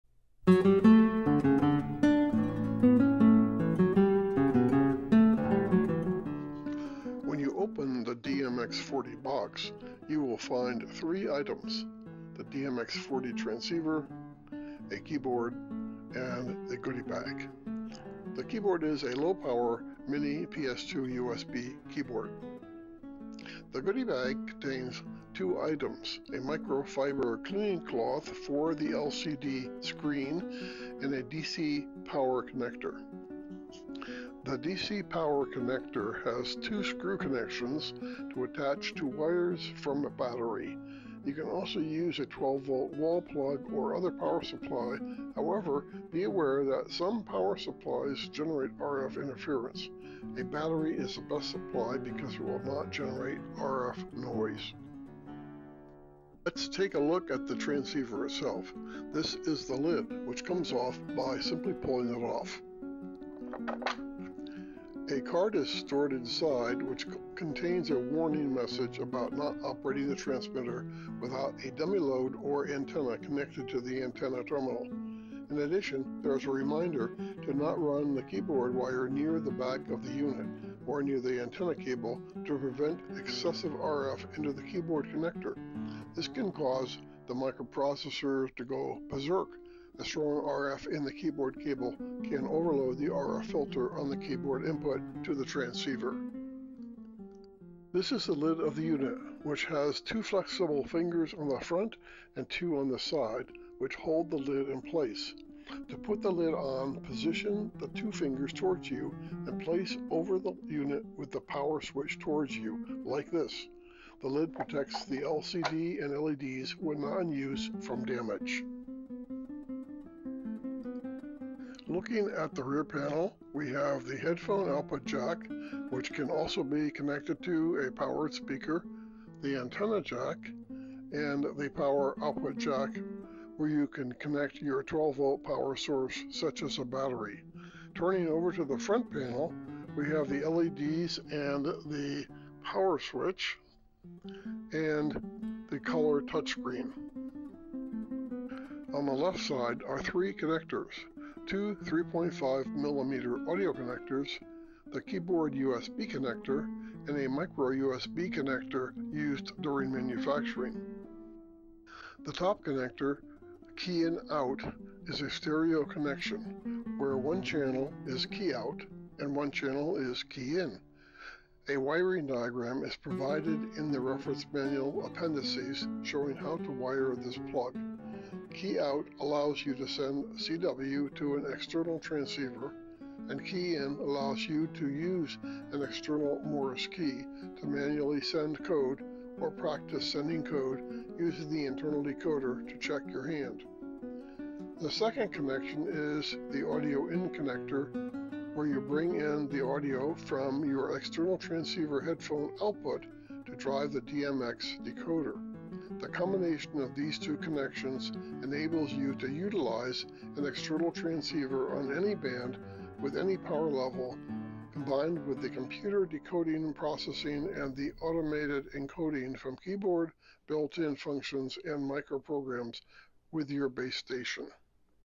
1. Introduction to the DMX-40 Morse Code Decoder & Converter Transceiver
This is the first in a series of 8 videos designed to give the viewer a overview of the operation and function of this very unusual transceiver.  Unlike most transceivers, the interface is a keyboard and a color touchscreen.  Its operation is unique - it has no direct competitor.  This series will help you understand what makes this product a very attractive option for a radio operator looking for an opportunity to operate on the HF bands with Morse Code without having to learn it first.  The DMX-40 will help you learn, if that is your goal, or it will allow you to operate on 40 meters (or any other band if you have a base station) with only a technician class license and no knowledge or experience of Morse Code.  It enables infrastructure-free communications alternative for preppers bound to a fallible repeater with their handheld VHF/UHF transceivers.  Fun to use, powerful 3-microprocessor automation and power!